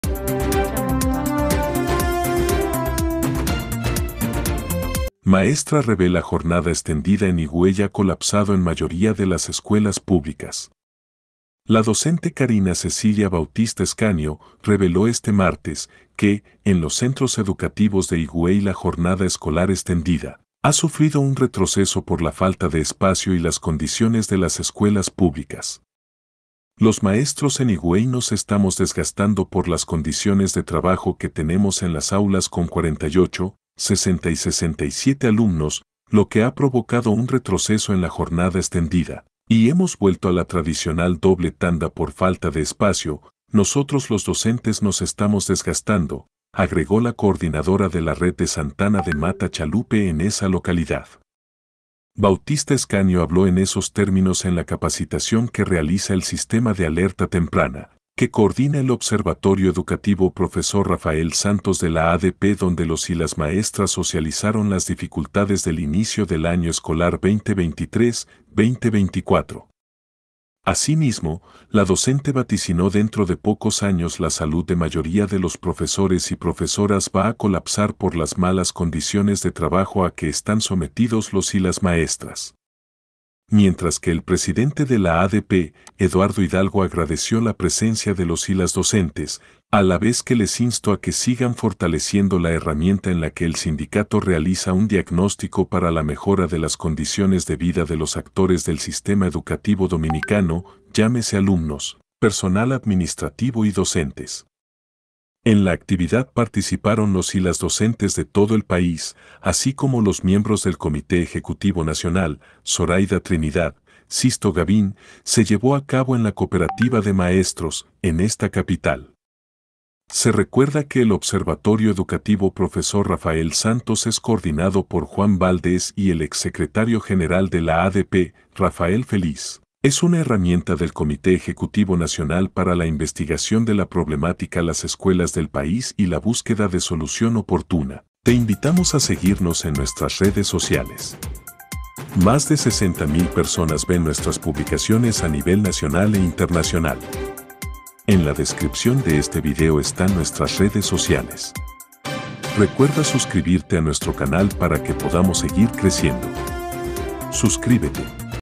Jornada Extendida en Higüey ha colapsado
La Docente Karina Cecilia Bautista Escanio, reveló este martes, que, en los centros educativos de Higüey la Jornada Escolar Extendida (JEE), ha sufrido un retroceso por la falta de espacio y las condiciones de las escuelas públicas.
“Los maestros en Higüey nos estamos desgastando por las condiciones de trabajo que tenemos en las aulas con 48, 60 y 67 alumnos, lo que ha provocado un retroceso en la Jornada Extendida, y hemos vuelto a la tradicional doble tanda por falta de espacio, nosotros los docentes nos estamos desgastando”, agregó la Coordinadora de la Red de Santana de Mata Chalupe en esa localidad.
Bautista Escanio habló en esos términos en la capacitación que realiza el Sistema de Alerta Temprana, (SATEP) que coordina el Observatorio Educativo Prof. Rafael Santos de la (ADP) donde los y las maestras socializaron las dificultades del inicio del año escolar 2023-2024.
Asimismo, la docente vaticinó dentro de pocos años la salud de mayoría de los profesores y profesoras va a colapsar por las malas condiciones de trabajo a que están sometidos los y las maestras.
Mientras que el presidente de la ADP, Eduardo Hidalgo agradeció la presencia de los y las docentes, a la vez que les insto a que sigan fortaleciendo la herramienta en la que el sindicato realiza un diagnóstico para la mejora de las condiciones de vida de los actores del sistema educativo dominicano, llámese alumnos, personal administrativo y docentes.
En la actividad participaron los y las docentes de todo el país, así como los miembros del Comité Ejecutivo Nacional, Zoraida Trinidad, Sixto Gabín, se llevó a cabo en la Cooperativa de Maestros, en esta capital.
Se recuerda que el Observatorio Educativo Prof. Rafael Santos es coordinado por Juan Valdez y el ex Secretario General de la ADP, Rafael Feliz; es una herramienta del Comité Ejecutivo Nacional para la investigación de la problemática las escuelas del país y la búsqueda de solución oportuna.

Canal oficial de los maestros dominicanos. Somos Magisterio Nacional RD.